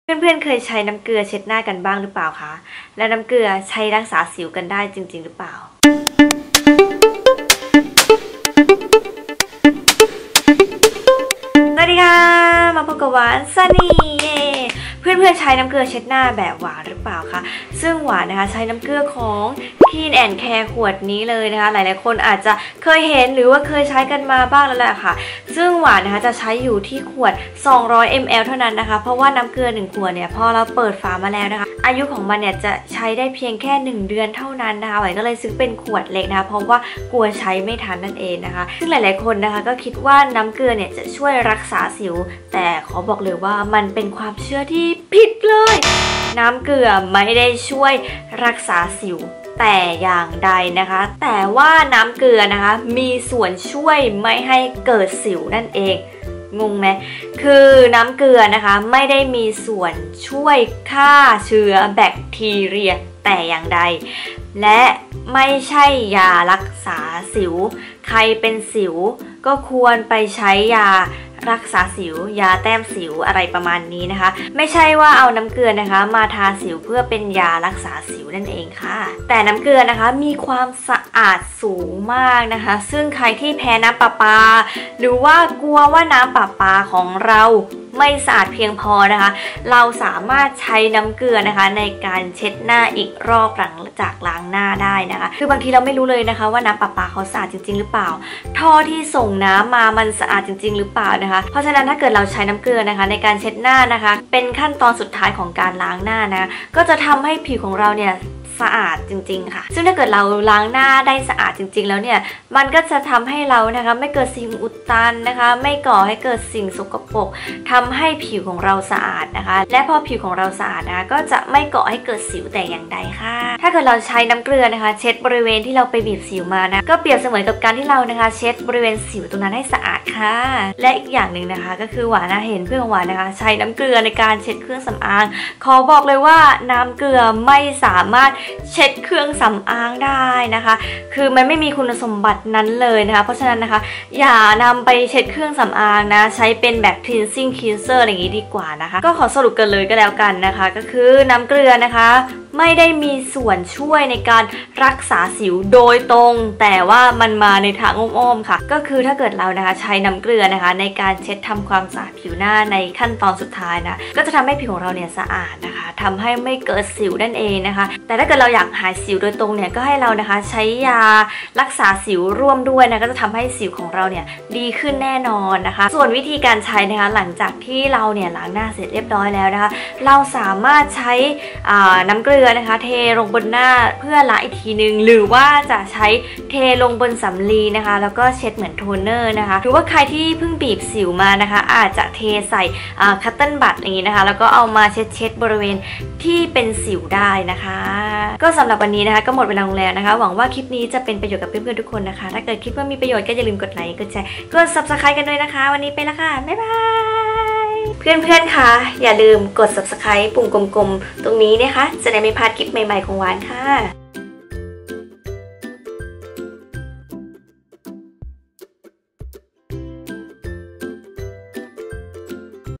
น้ำเกลือใช้รักษาสิวได้จริงเหรอ?? | WaanSannie
น้ำเกลือใช้รักษาสิวได้หรือไม่ อยากรู้ไปดูกัน
Happy Happy นะคะ ♥ ขอบคุณที่เข้ามาคอมเม้นต์นะคะ ♥
มีอะไรคอมเม้นต์คุยกันได้ใต้คลิปนี้เลยจ้า จุ๊ฟๆ